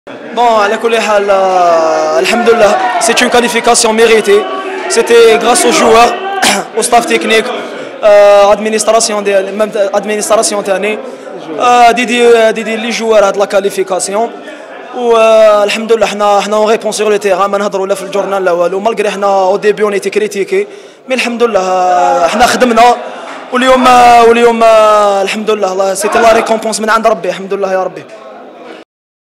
JSK (1-0) Stade Malien | 🎥 Aziz Benabdi revient sur la qualification
JSK (1-0) Stade Malien  | 🎥  Aziz Benabdi revient sur la qualification de son équipe aux poules de la coupe de la CAF.

💛 Jeunesse Sportive de Kabylie 💚